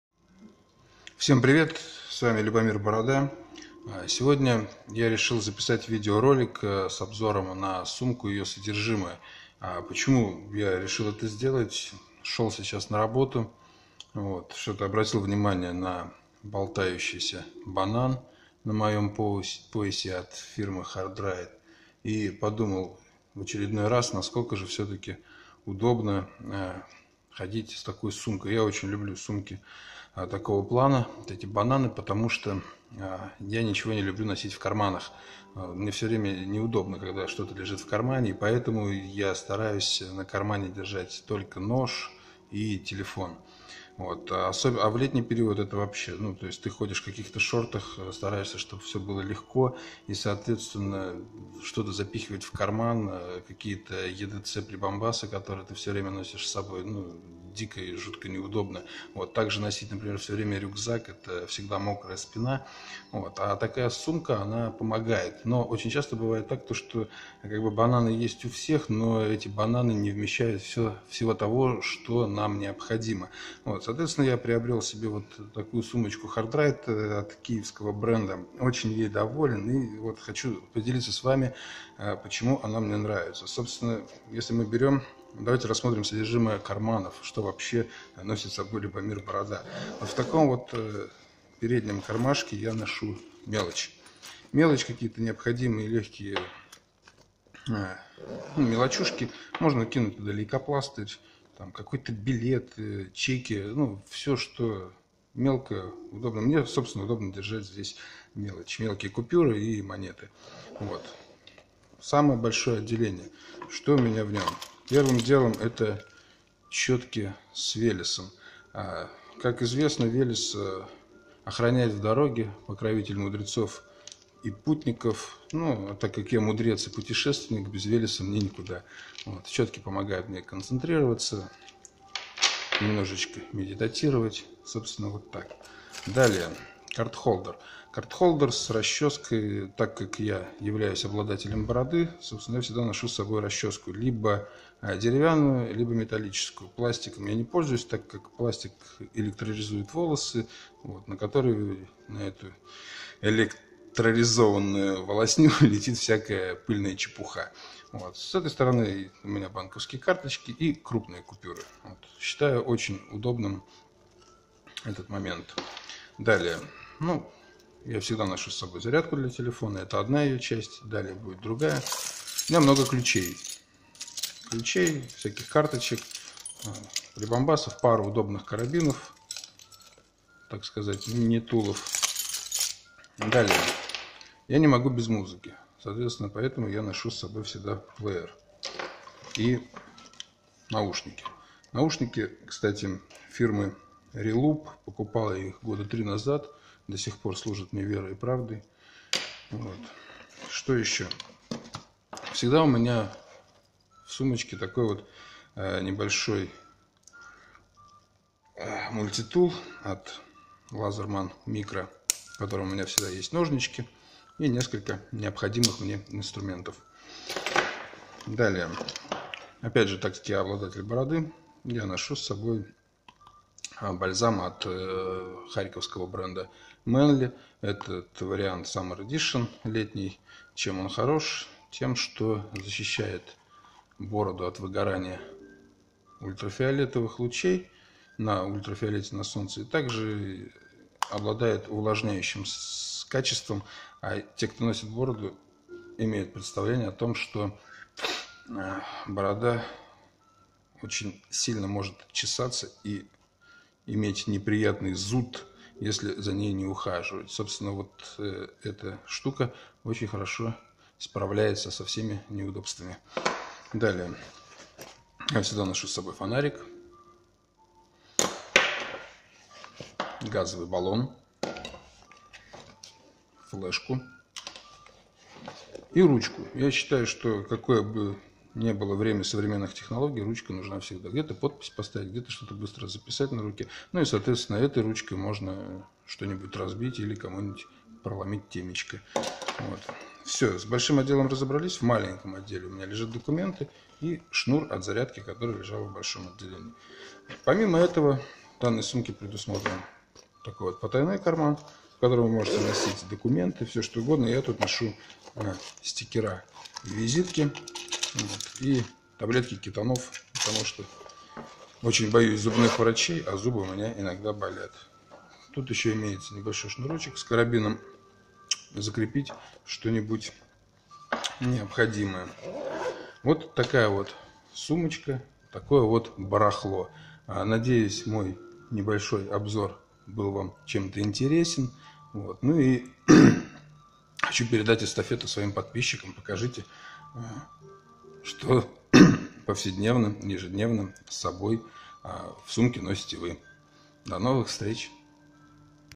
Что в моем waist bag HARDRIDE?
Сумка "банан" - waist bag HARDRIDE. Почему я считаю ее незаменимым edc атрибутом? В этом видео я постарался раскрыть этот вопрос. Ролик снимал на камеру смартфона, без подготовки, поэтому некоторые речевые обороты в стиле Кличко ))
Кто готов принять эстафету и показать содержимое своей сумки?))